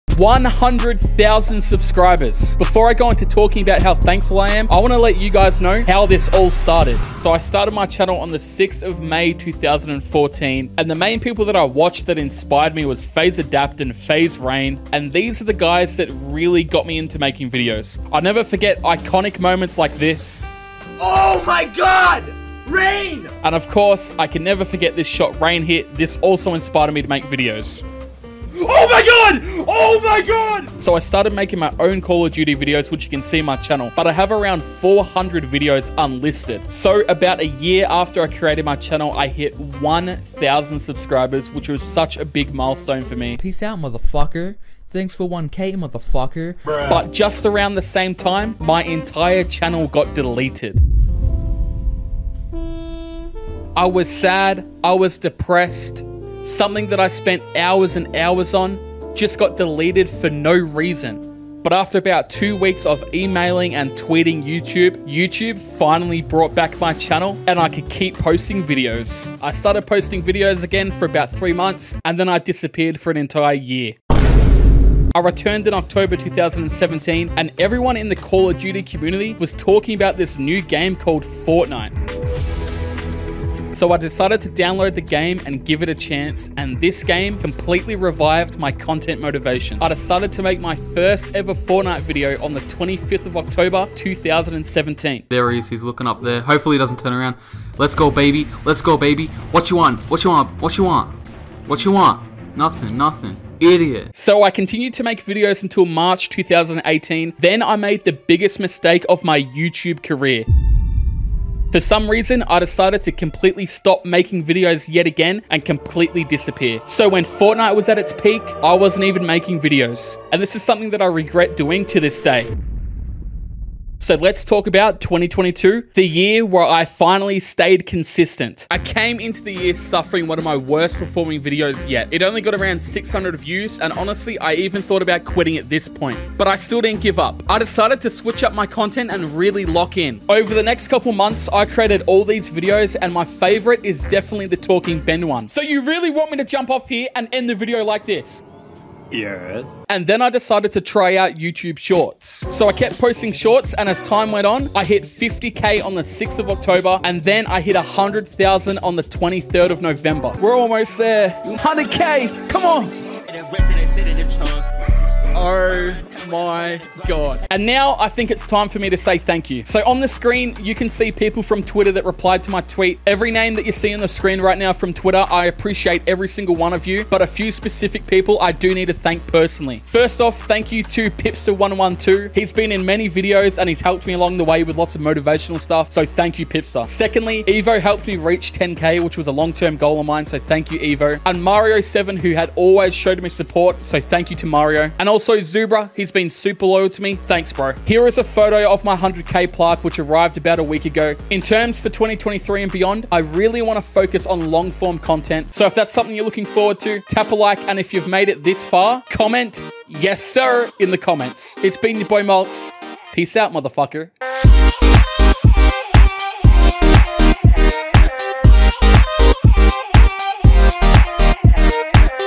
How I Reached 100,000 Subscribers...
Thank you so much for 100K
I have been dreaming of this moment since I was 13 years old.

I have been so inconsistent over the time I've spent on YouTube, at many times I thought I would quit, but I never did. I kept coming back and trying and eventually I reached my goal, and you can do it too.

Believe in yourself and never give up, and if you're dealing with stuff IRL, remember all emotions are temporary and never make a permanent decision based off temporary emotions.

Thank you to the OGs that have stuck by me during my inconsistency, I appreciate all of you,